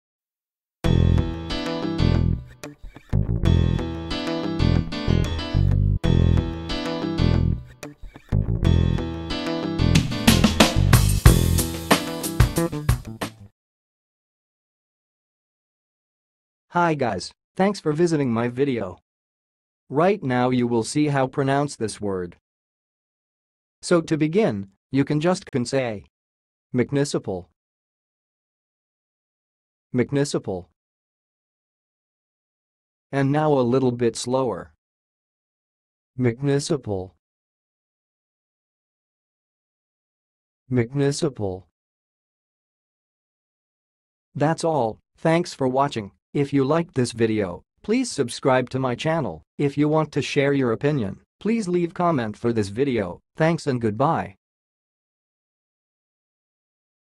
MUCNICIPAL - HOW TO PRONOUNCE IT!?
Watch how to say and pronounce "mucnicipal"!